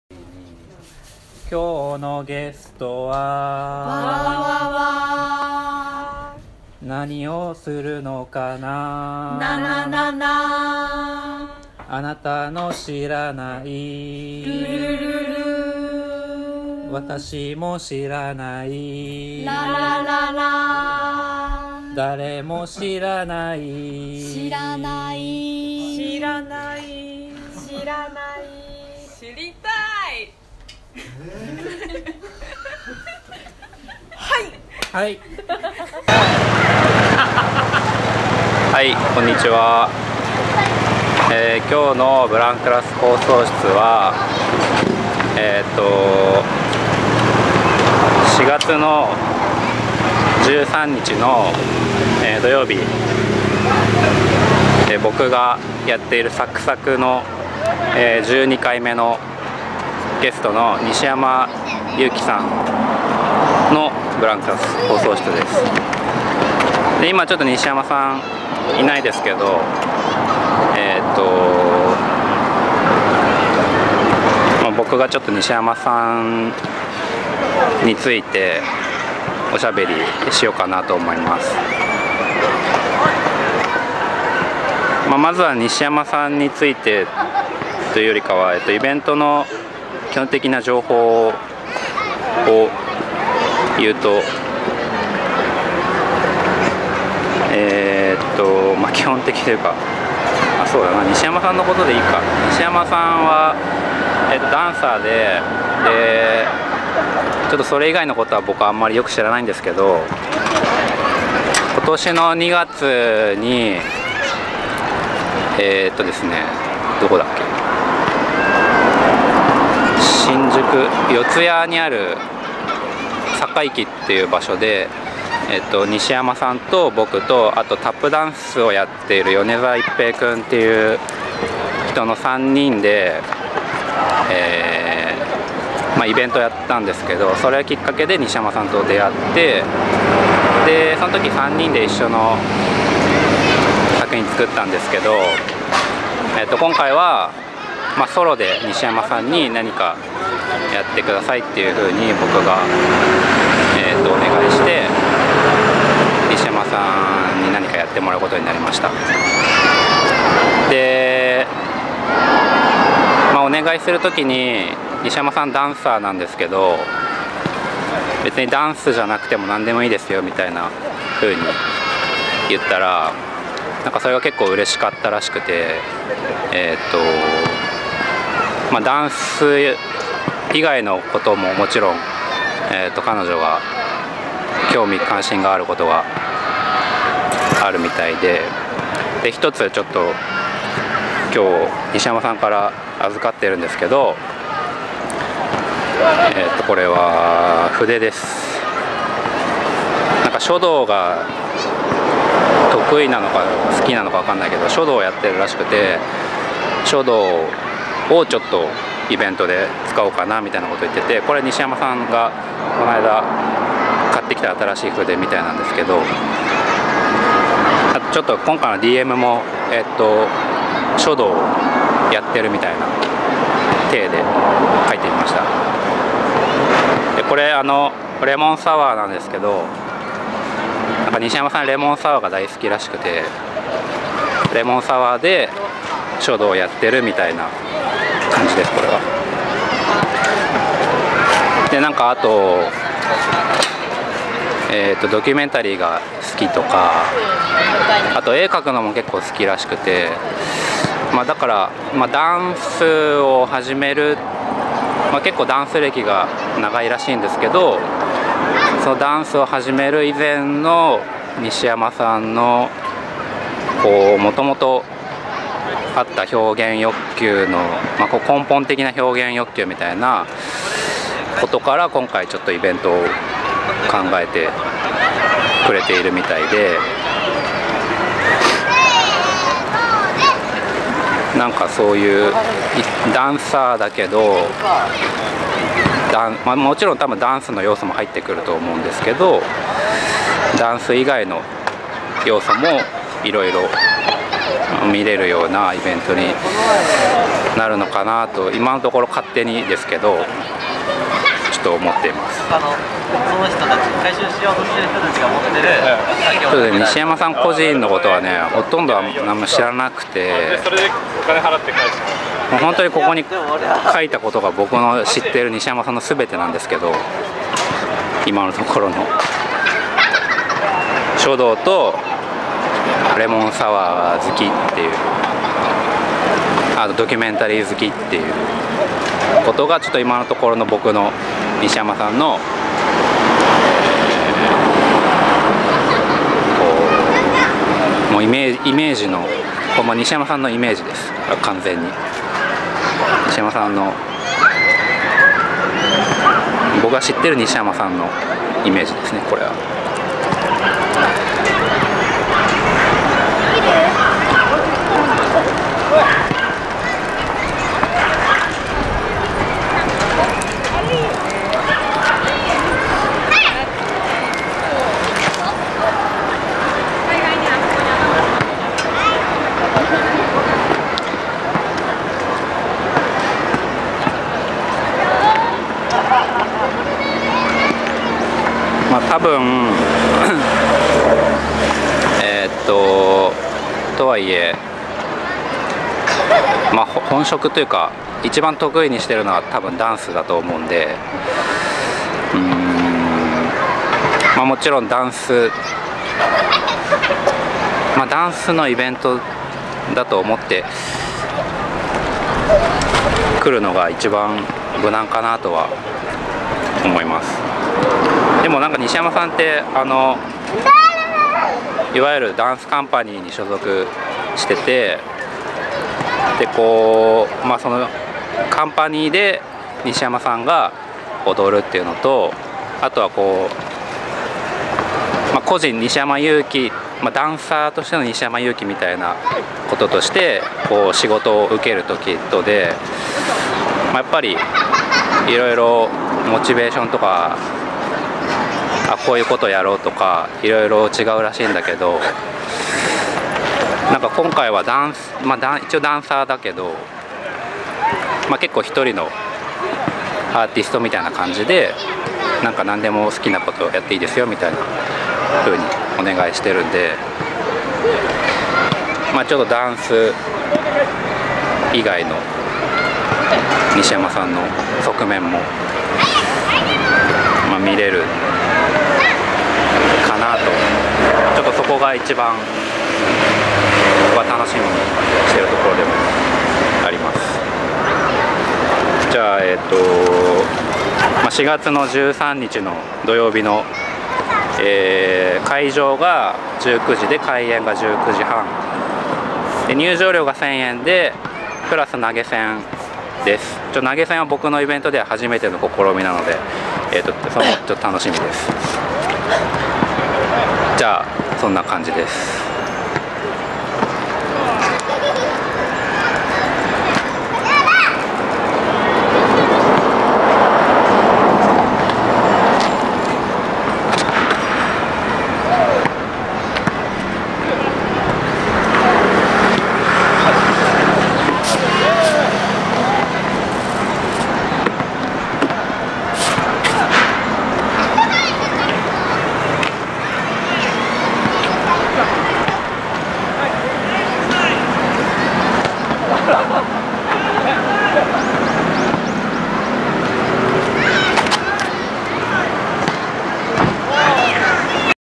2019/4/8/Yuki NISHIYAMA/blanClass broadcasting
2019/4/8/西山友貴/blanClass放送室

4月13日（土）のSakSak#12、ゲストの西山友貴さんのblanClass放送室を公開しました。
僕が、西山さんと今回のイベントについてひとりで話しています。西山さんは僕の後ろで踊っていて、ちょっとずつ大きくなってきたりして、最後は西山さんに絡まれます。

SakSak#12　西山 友貴
Re:start｜ダンス、セルフドキュメンタリー

企画の野本くんから「なんでも好きなことやっちゃってください」とお誘いをうけました。ダンスと出会って26年。時が経つのは早いなーなんて考えていたら、幼い頃に自分が好きだったモノやコトについて色々思い出しました。
2019年4月13日、この日、この場所から始めてみることにしました。つらつらと描きながら。思いのまま。

出演：西山 友貴／企画：野本 直輝
入場料：1,000円+投げ銭
会場：blanClass（横浜市南区南太田4-12-16）
アクセス：京浜急行「井土ヶ谷」駅の改札出て正面の信号わたりすぐを左折、一つ目の交差点を右折、二つ目の角を左折、三井のリパーク後ろ、blanClass看板がある細い段々を上がって右の建物2階

西山 友貴　Yuki NISHIYAMA
ダンサー。筑波大学大学院修了。09 年、文化庁海外研修員として一年間 NY に留学。
平山素子、北村明子、山田うん、Inbal Pinto & Avshalom Pollakなど国内外の振付家の作品に参加。近年では自身のユニット〈Atachitachi〉の活動をはじめ、様々なアーティストとのコラボを展開。ワクワクすること模索中。

SakSak
SakSakでは、誰かが発する表現を手掛かりに、その先を一緒に考えながら、その思考を交換することができる場の可能性を模索します。そのために取り敢えず、粗くてもろい、隙間だらけの場を想像してみる。（野本直輝）